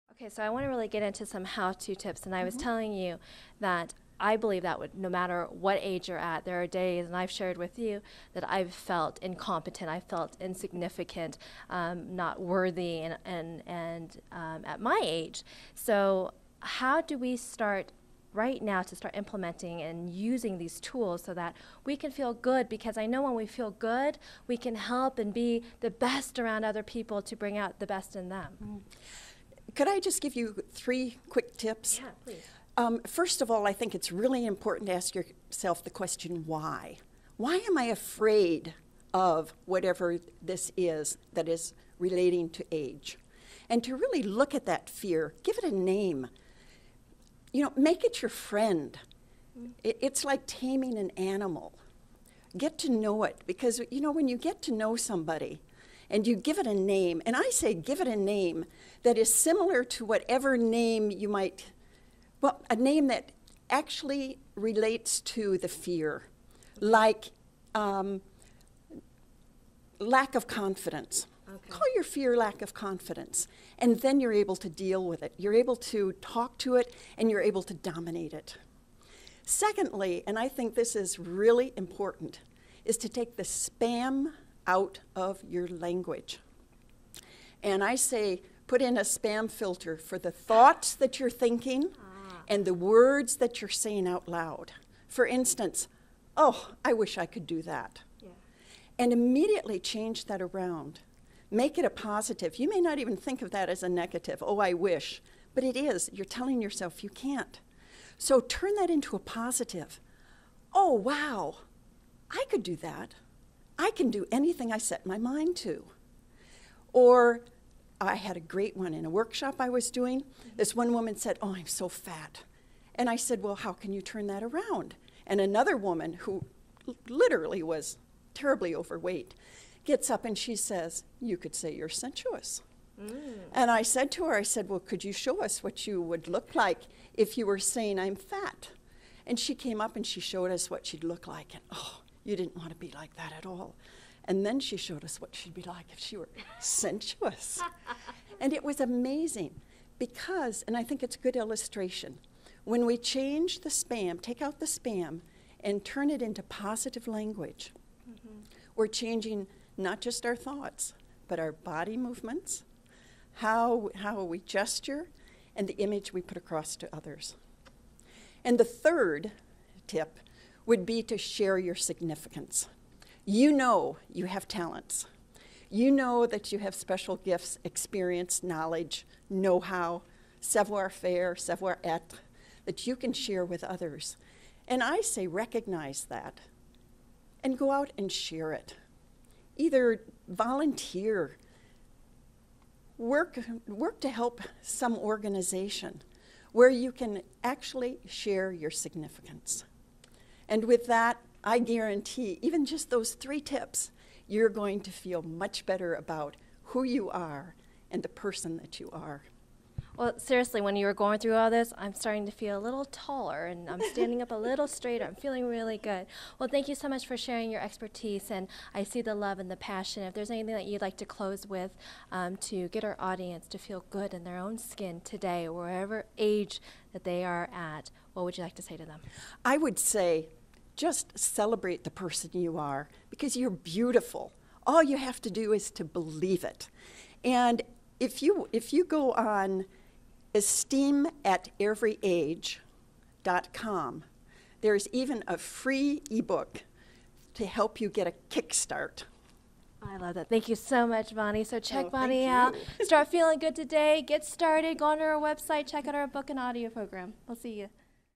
Hangout with Bonnie Lou Fatio, Maria Ngo & Ray DuGray on the Red Carpet (Part II) | Power of 3 Event
| Join Maria Ngo and Ray DuGray on the red carpet as they hangout with Bonnie Lou Fatio for the Power of 3 event in Las Vegas.